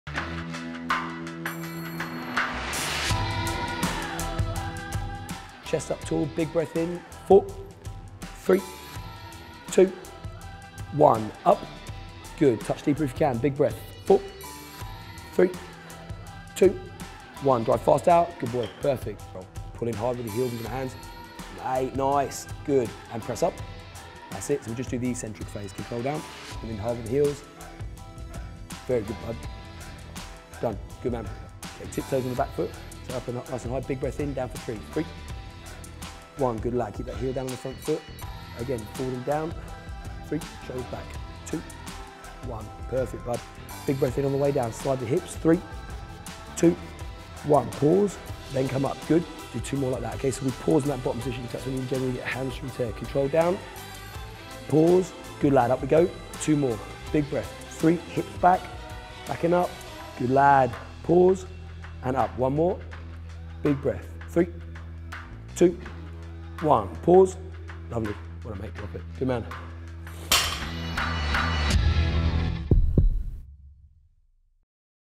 MARO ITOJE | TRAINING SESSION
Maro Itoje has provided a short Maximuscle video to showcases his training to get in peak condition. Watch here.